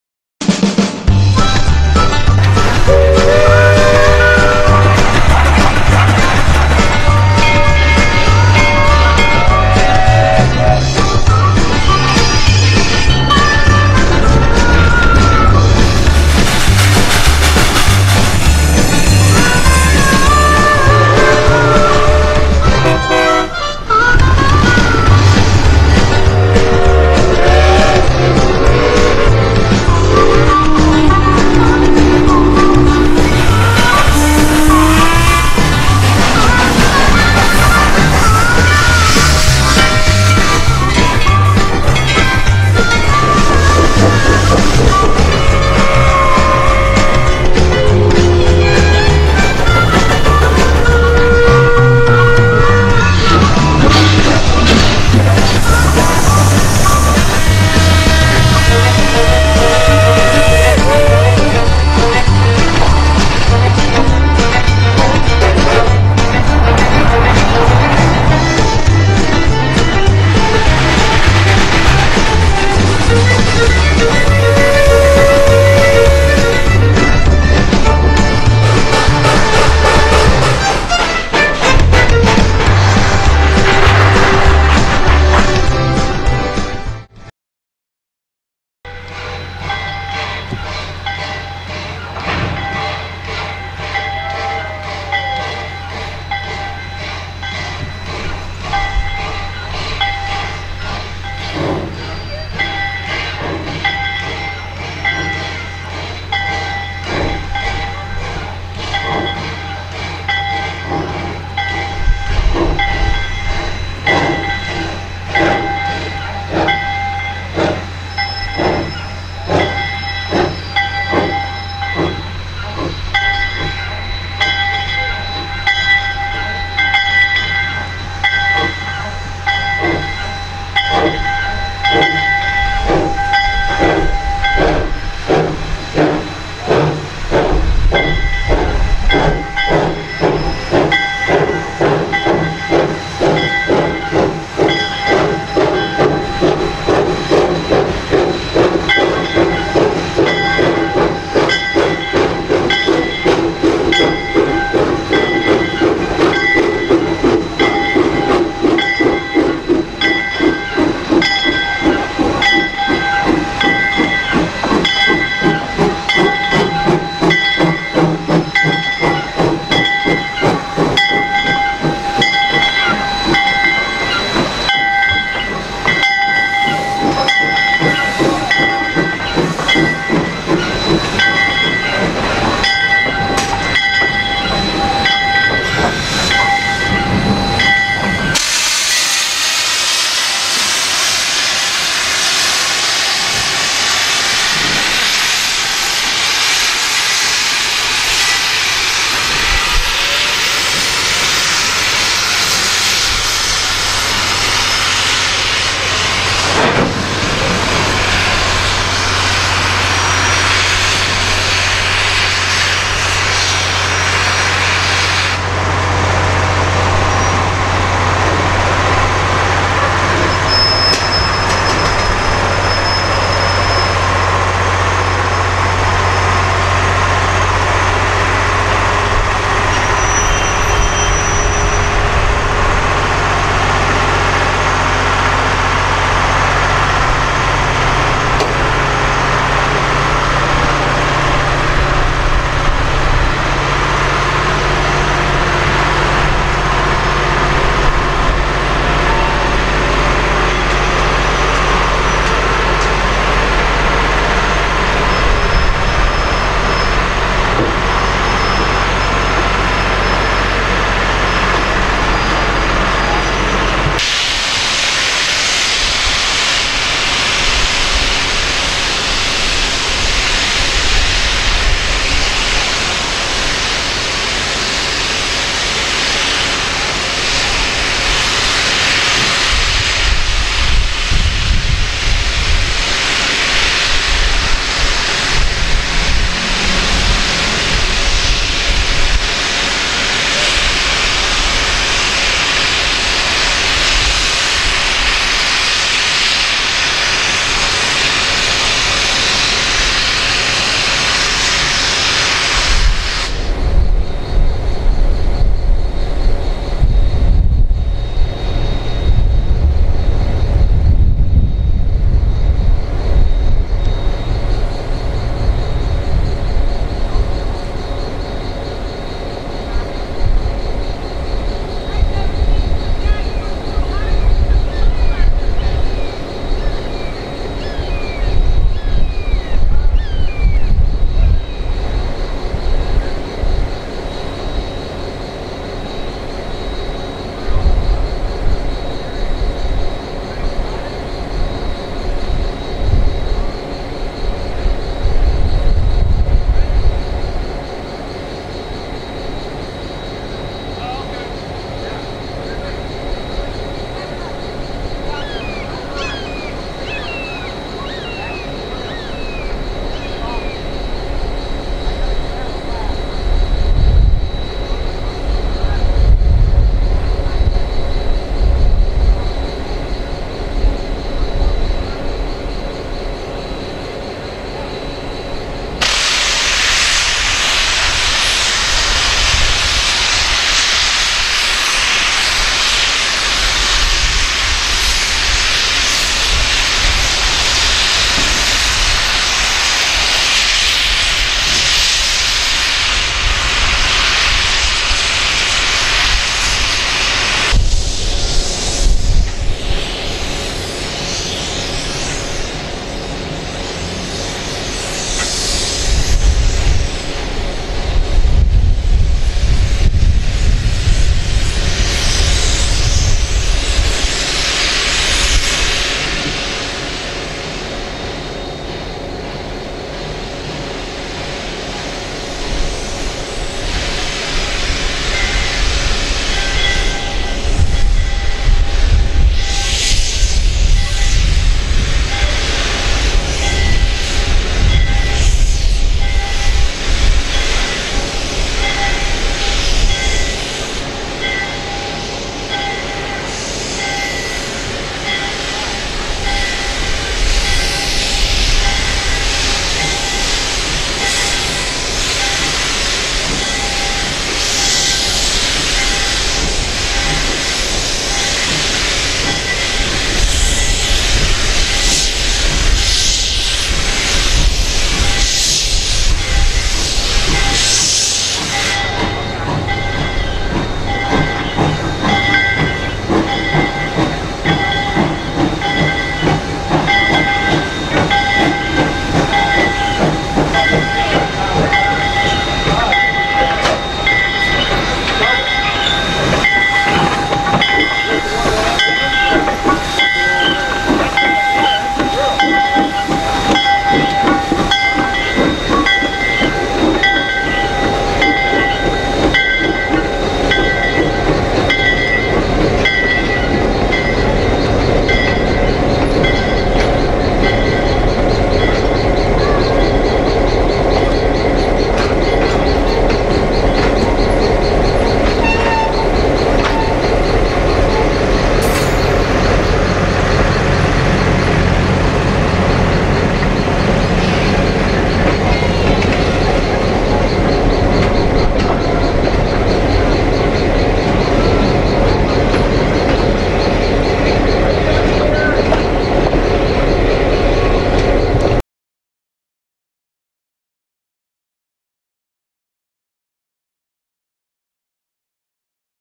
1906, Charleston Lumber 2-4-0 No. 3, on display in Archer FL, 2001
2-4-0 in Archer FL, 2001